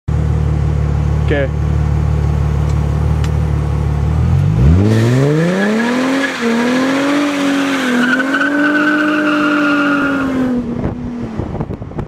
4.0 v6 mustang launch
2006 v6 mustang, X-Charger Supercharger(m90), 36lb injectors, 10psi(2.8 pulley), Borla exhaust, SCT Liveiwre Tuner w/ BAMA Race 93oct, LCA's and UCA